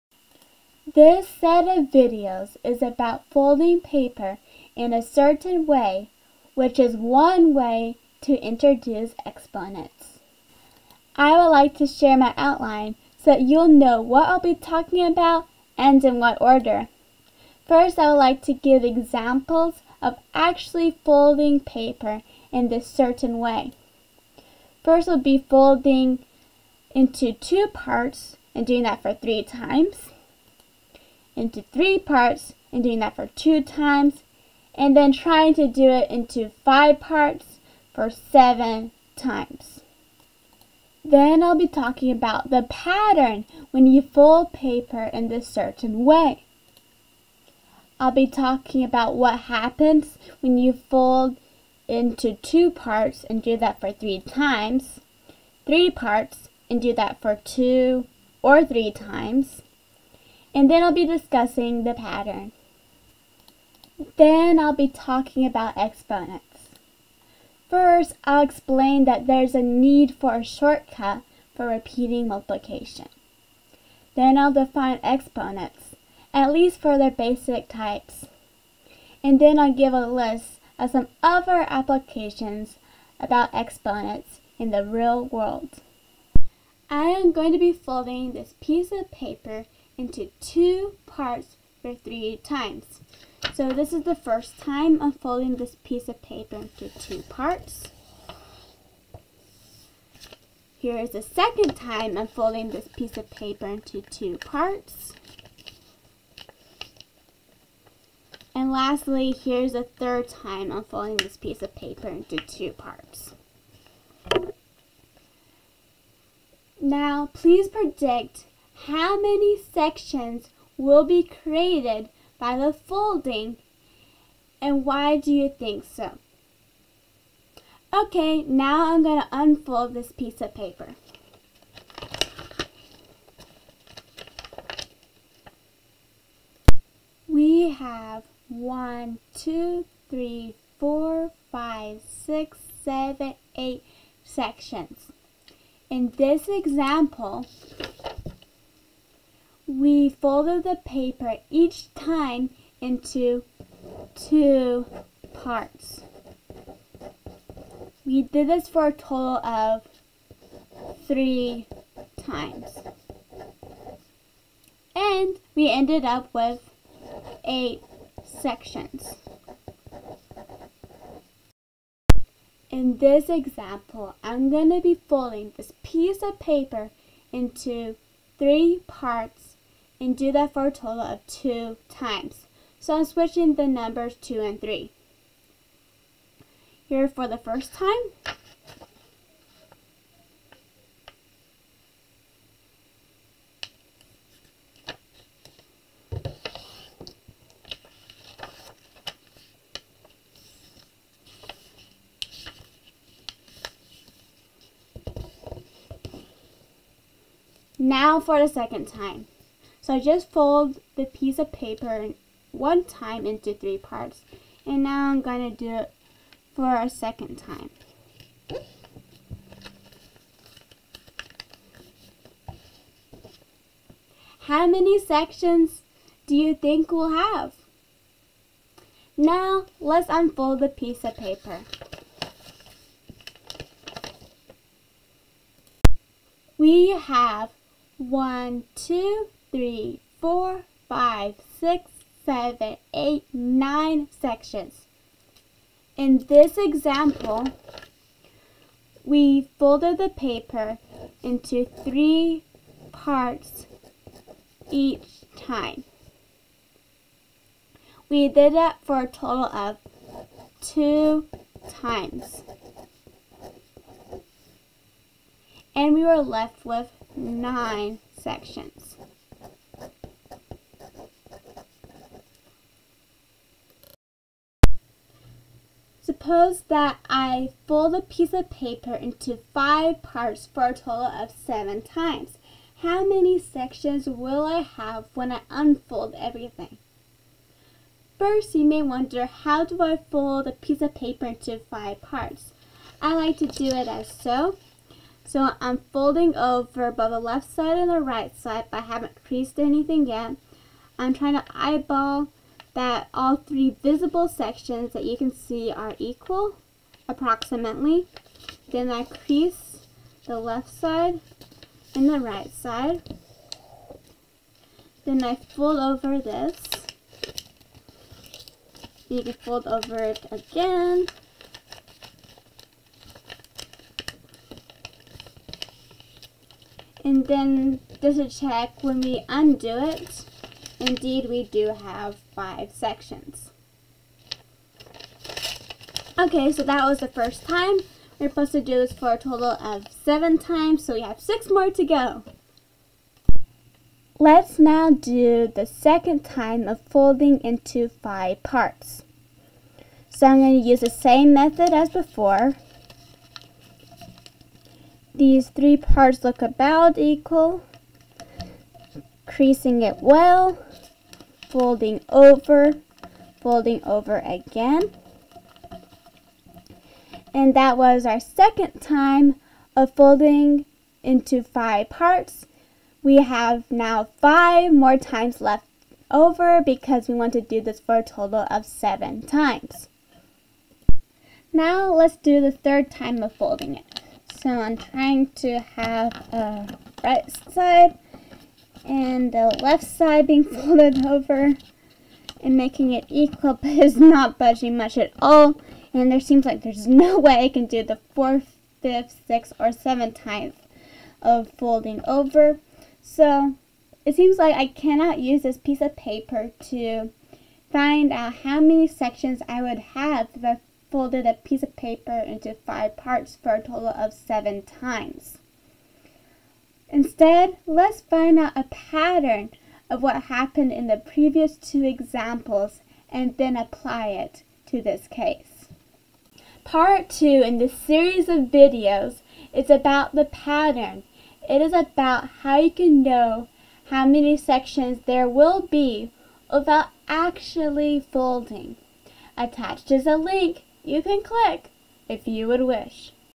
Folding Paper to Introduce Exponents (Part 1: Examples of Folding Paper)
This set of videos is about one way to introduce exponents. Also, this set of videos explains why folding paper in this certain way is related to exponents.

To note, if you fold paper zero times, you have no creases but only the edges as boundaries. Therefore, you have 1 section. So, this example could be one way to show that n ^ 0 = 1, where n is any natural number.